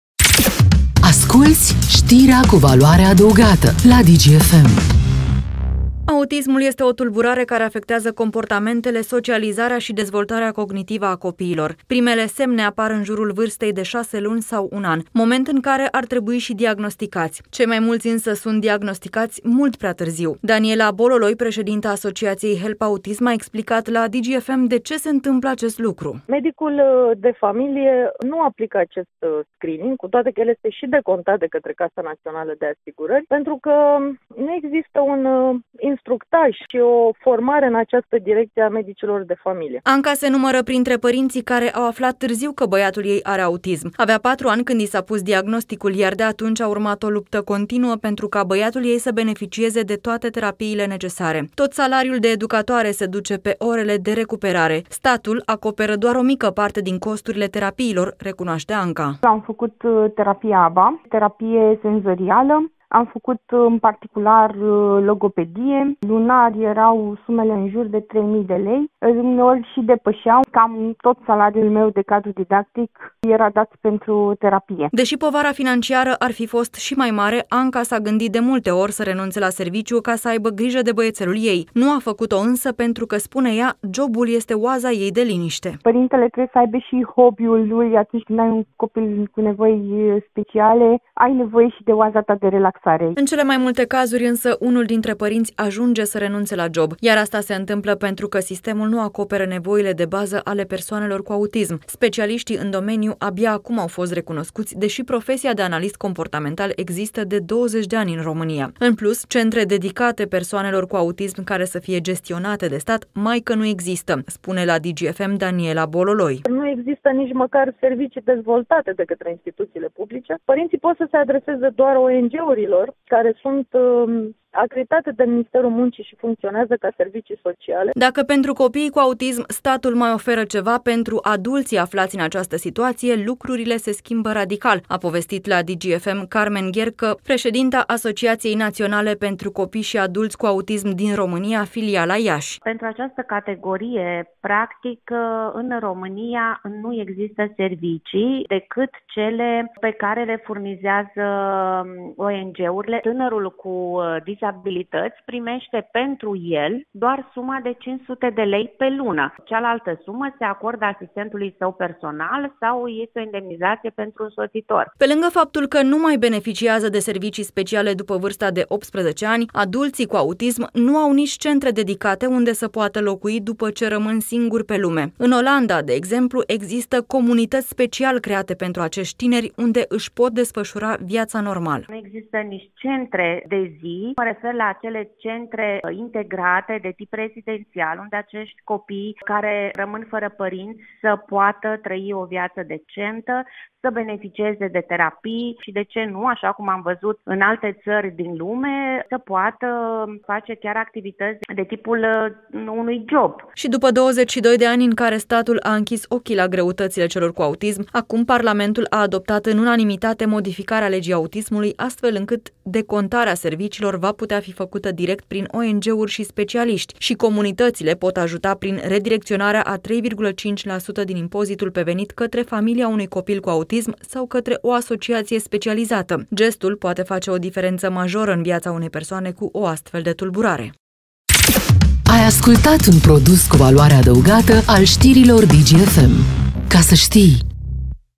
Știrea cu valoare adăugată - Copiii cu autism
1 din 44 de copii este diagnosticat cu tulburări din spectrul autist până la vârsta de 3 ani.
În România se estimează că trăiesc 40 de mii de persoane cu autism, iar dintre acestea aproximativ 15 mii se află în evidența statului. Adică primesc o alocație și indemnizație de însoțitor pentru cei care au grijă de ei.
Mulți consideră că autismul este o condamnare, însă, copiii afectați de autism pot avea un viitor și o dezvoltare aproape de normalitate. Pentru asta, însă, este nevoie de terapie specializată. Terapiile de bază ajung și la o mie de euro pe lună, sumă pe care statul nu o acoperă nici măcar pe jumătate.
Cum se descurcă în aceste condiții părinții care au copii cu autism sau adulții aflați în această situație aflați de la Alexandra Grigoriță.